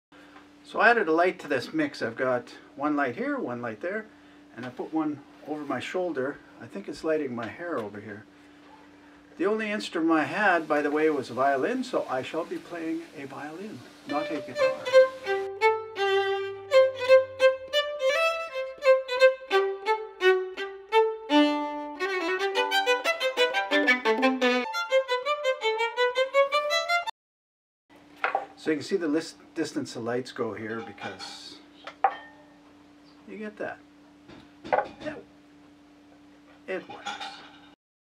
Testing Black Background
Testing out the ability to video a solid black background.  Do need to add secondary lighting but this is my first attempt.